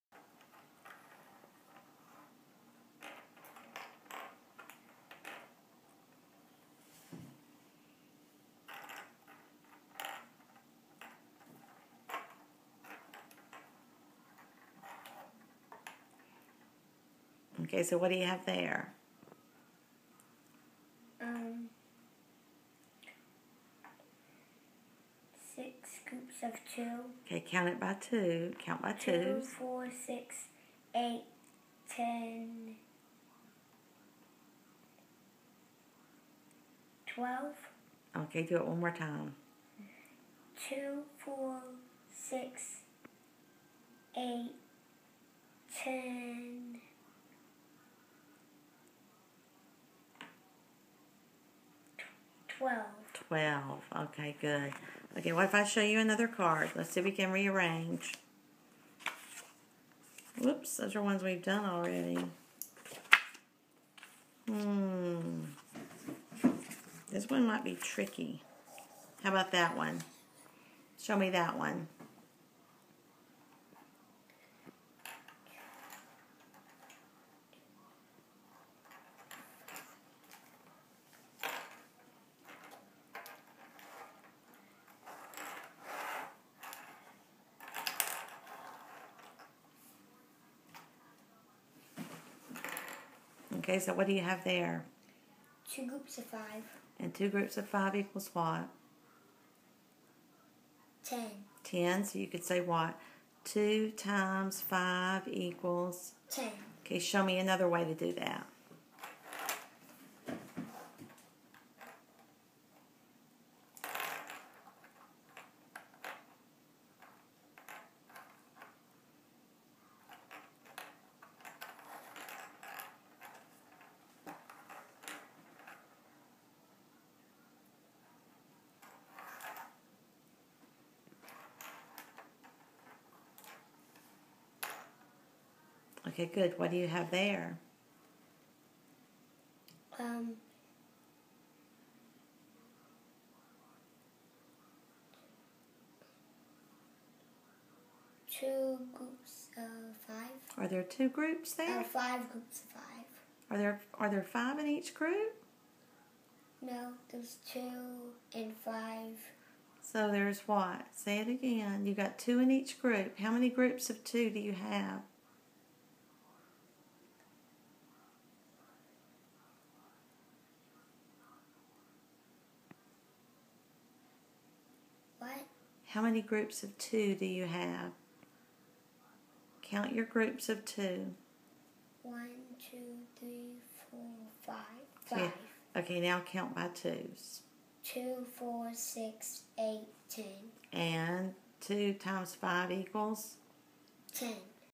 Teaching Multiplication with Manipulatives
Teaching multiplication using manipulatives. The child needs to use objects to create arrays in order to understand multiplication. This is an example of this teaching method.